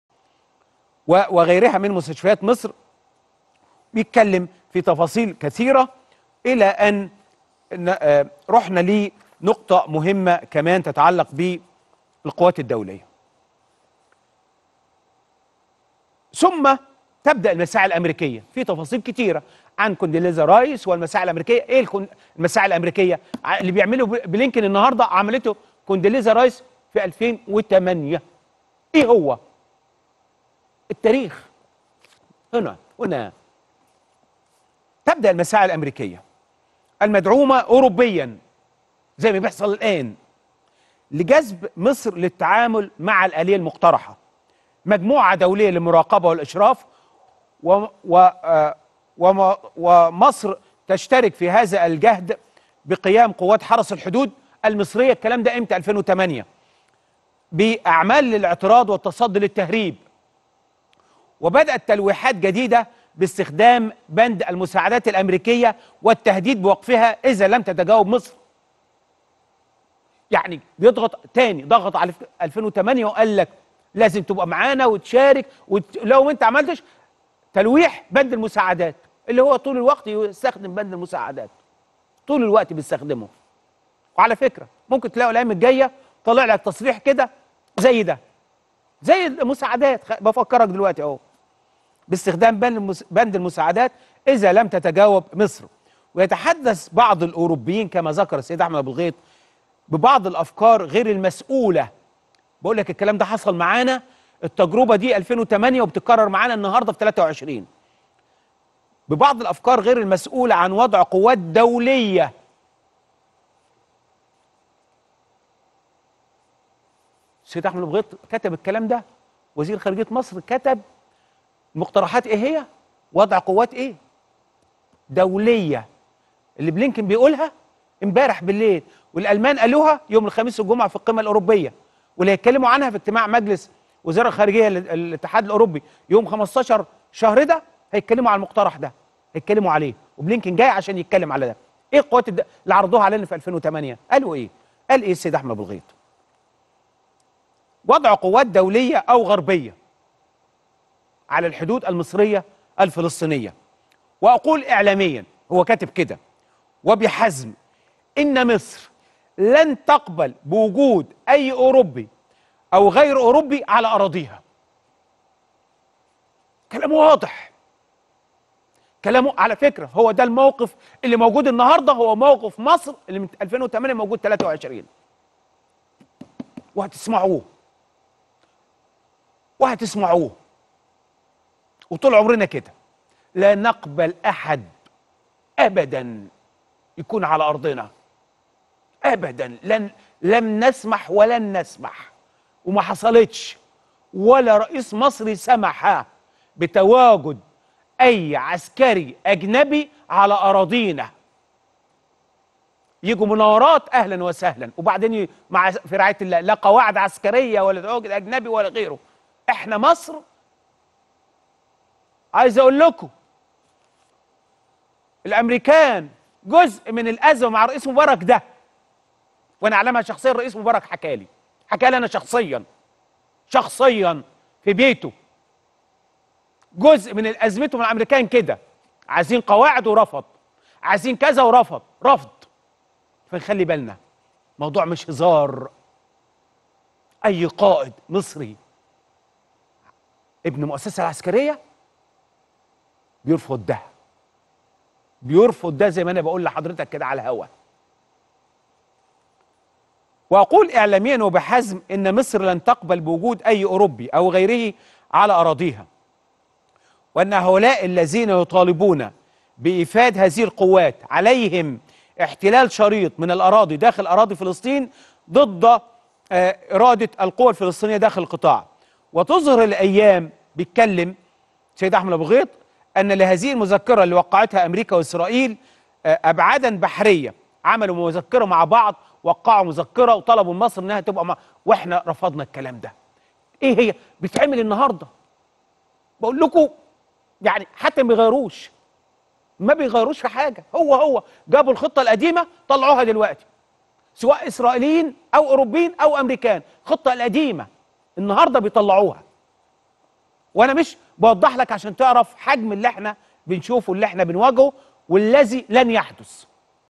مبارك قالهالي شخصيًا.. أحمد موسى يعلن مفاجأة بشأن خلاف الرئيس الراحل مع أمريكا
ElBaladOfficial#
ElBalad_Official#